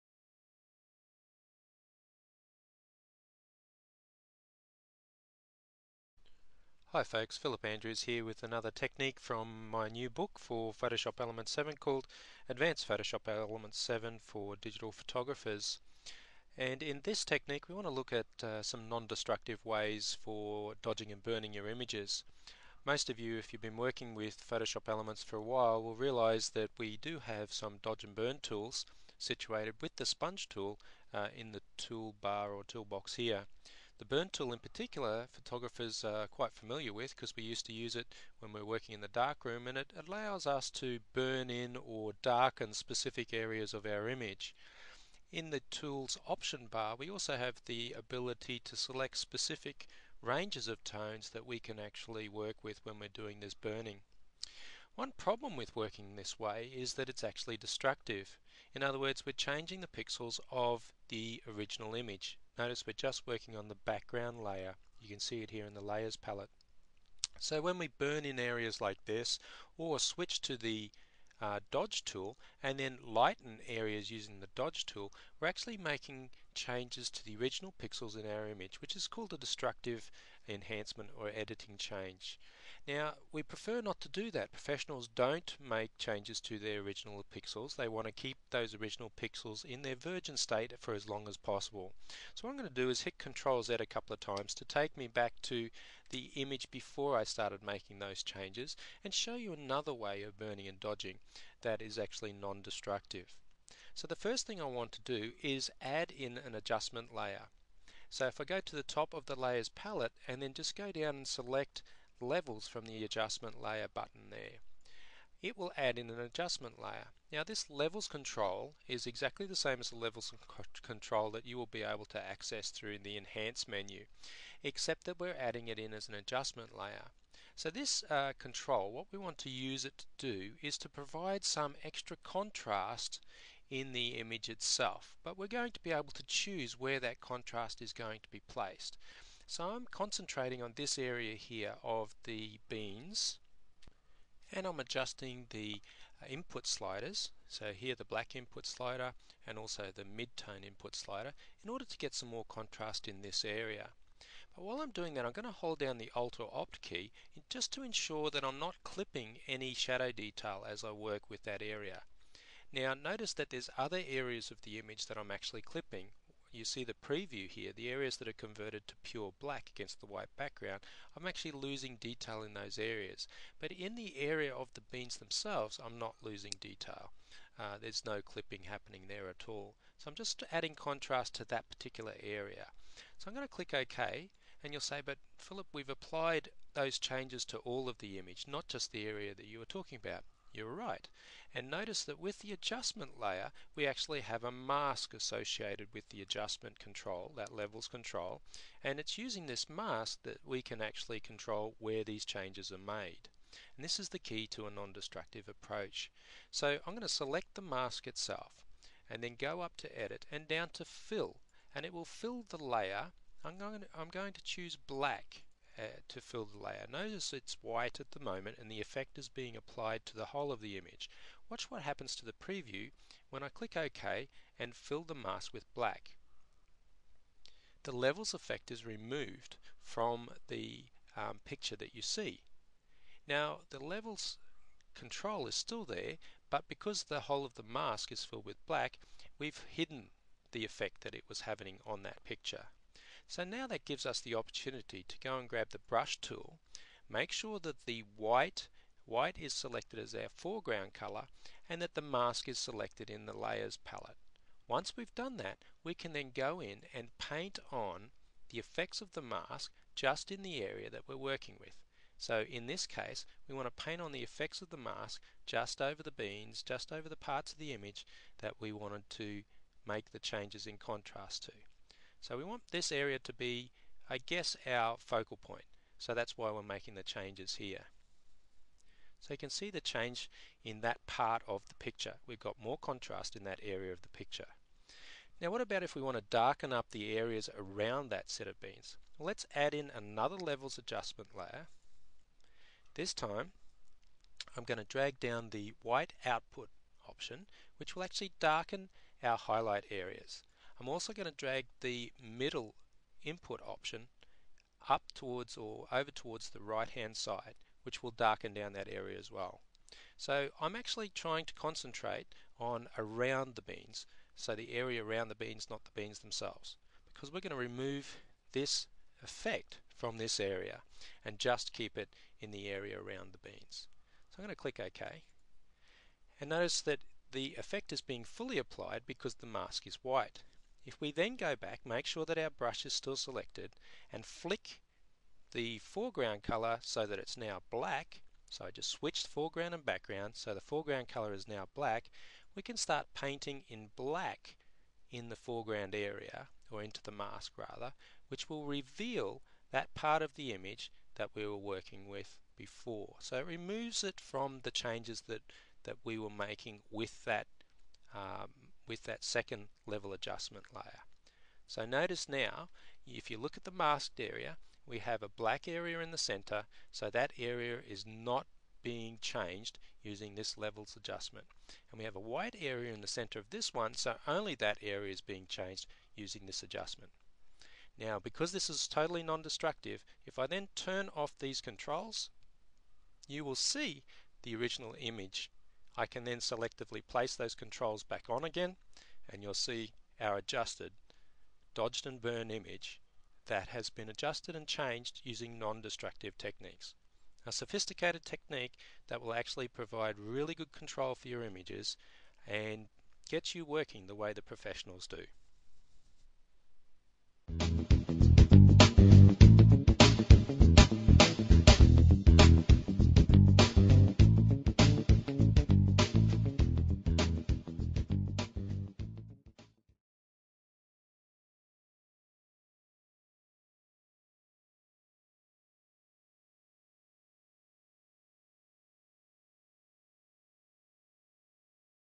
Photoshop Elements: Advanced Dodging and Burning
Elements offers a number of ways to darken or lighten entire photos either automatically or with the dodge and burn tool. Learn more in this video overview.